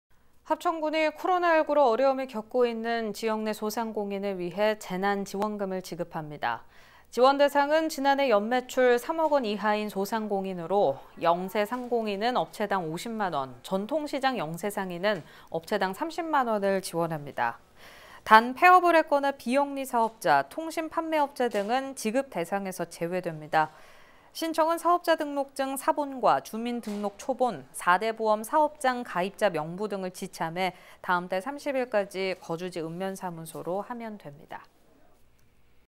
합천군, 소상공인에게 합천형 재난지원금 지급
합천군도
코로나19로 어려움을 겪고 있는
지역 내 소상공인을 위해
재난지원금을 지급합니다.